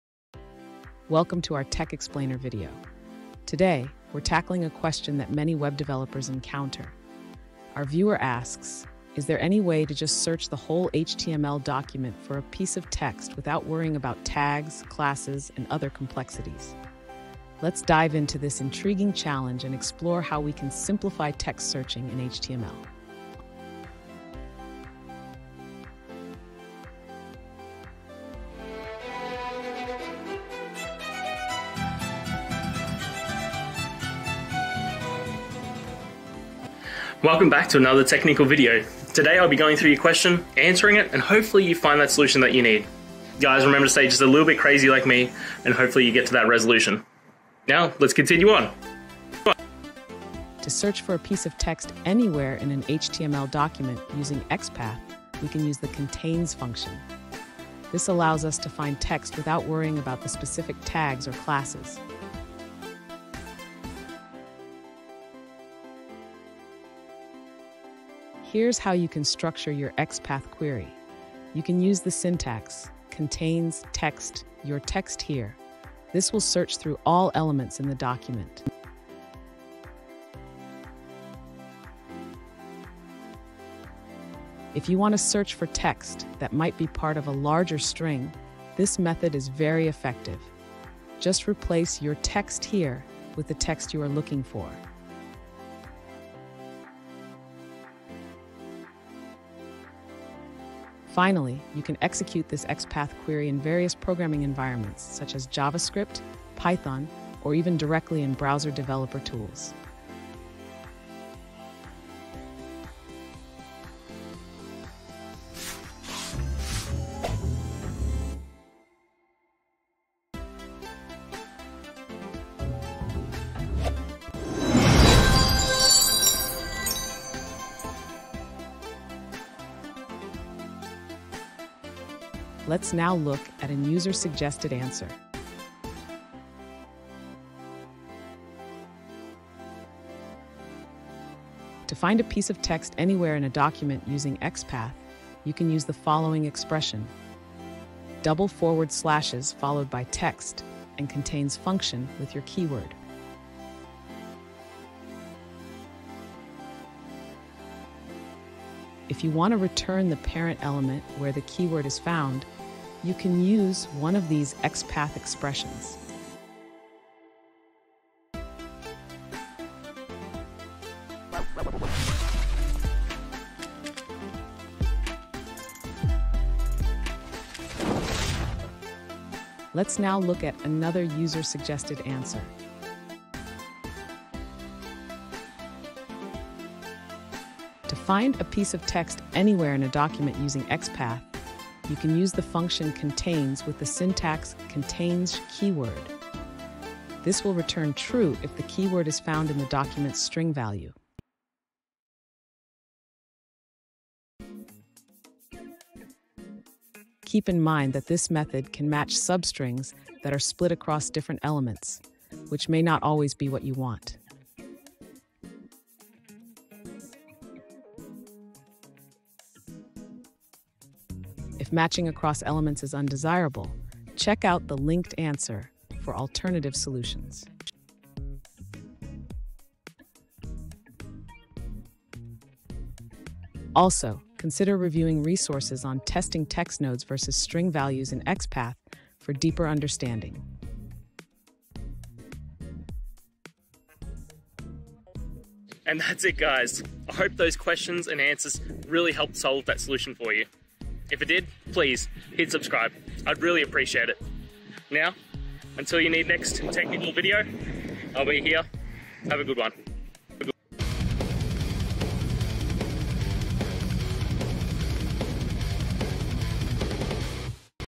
How to Use XPATH to Find Text Anywhere in an XML Document
In this video, we’ll explore the powerful capabilities of XPATH, a language designed for navigating and querying XML documents. Whether you're a beginner or looking to enhance your skills, you'll learn how to effectively locate text within complex XML structures. Join us as we break down the essential techniques and tips for mastering XPATH, enabling you to extract the information you need with ease.

Today's Topic: How to Use XPATH to Find Text Anywhere in an XML Document

Related to